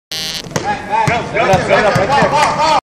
Morris"Momo"Rentie,III Jumpball & a free throw!(1)
Momo out-jumps the kid, then hits a smooth free-throw!